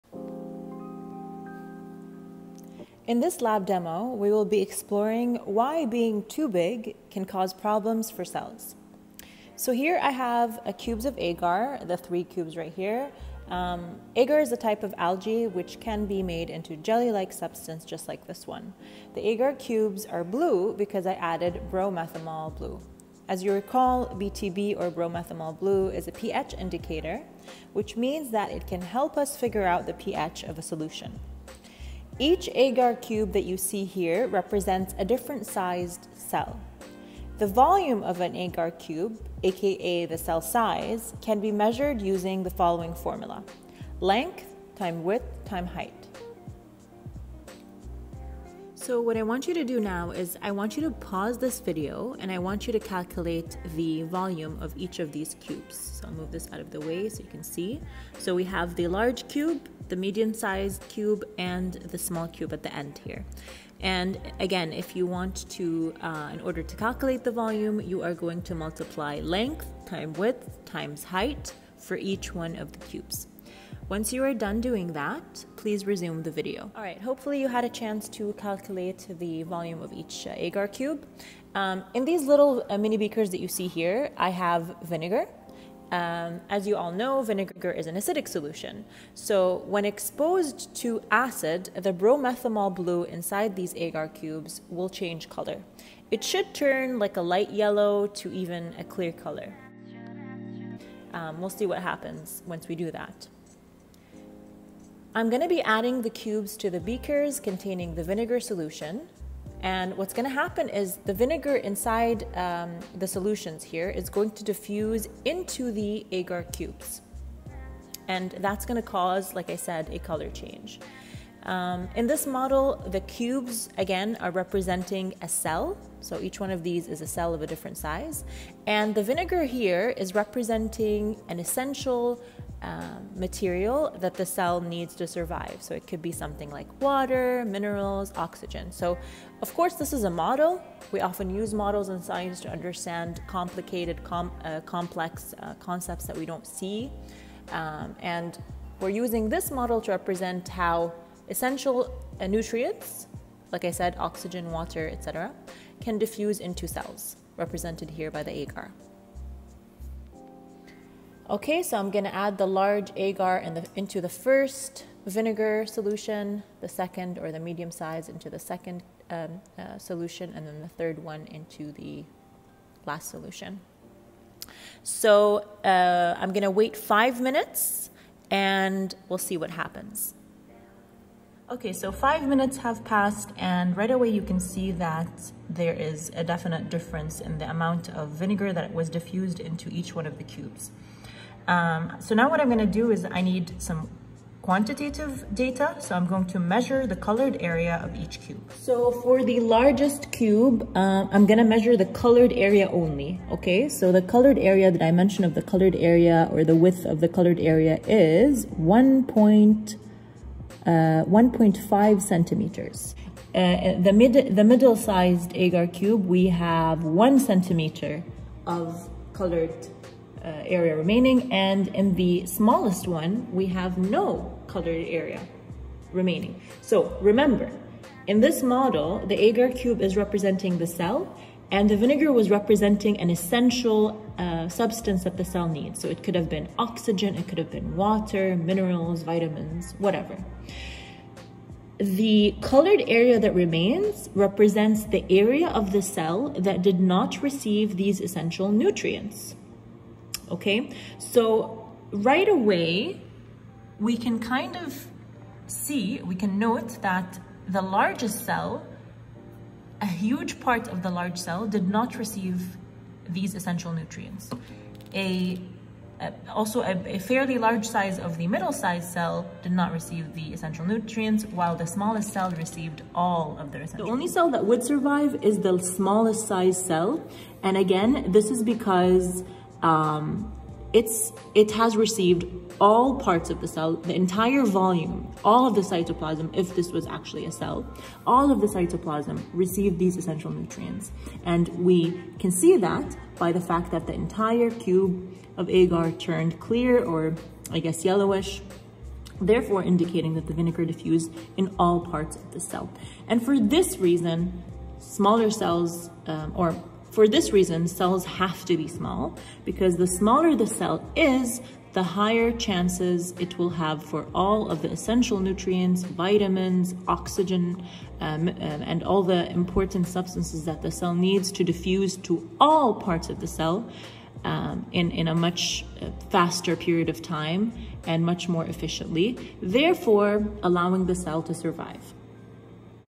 Why is Cell Size Limted?
Learning Targets Covered: I can explain why being too big causes problems for cells